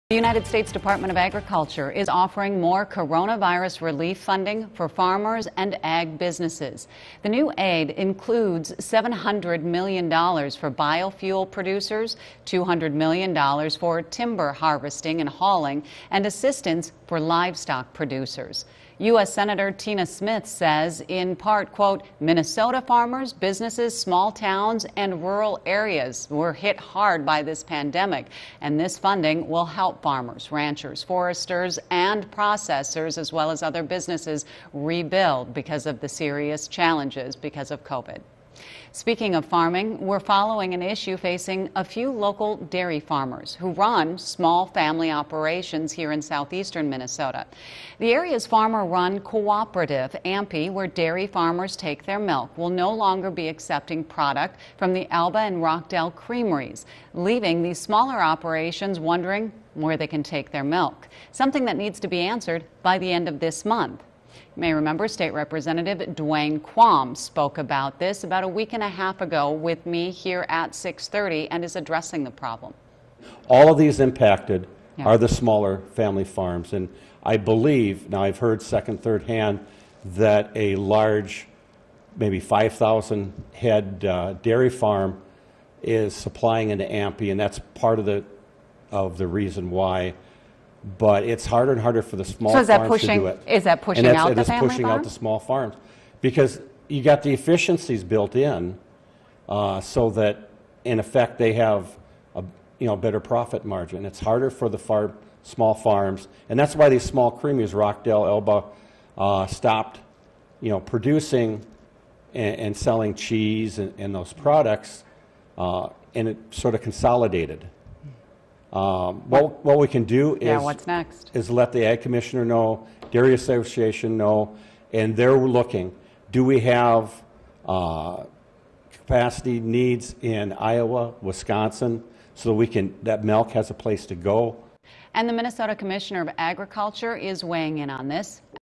Dairy Industry issues
(ABC 6 News) - The United States Department of Agriculture is offering more coronavirus relief funding for farmers and agricultural businesses.

The new aid includes 700-hundred million dollars for biofuels producers, two hundred million dollars for timber harvesting and hauling, and assistance for livestock producers.

We're following an issue facing a few local dairy farmers who run small family operations here in southeast Minnesota. The area's farmer run cooperative where dairy farmers take their milk will no longer be accepting product from the Elba and Rock Del creameries leaving these smaller operations wondering where they can take their milk... something that needs answered by the end of the month.

Minnesota Representative Duane Quam is working on this issue and spoke at length last week about the issue.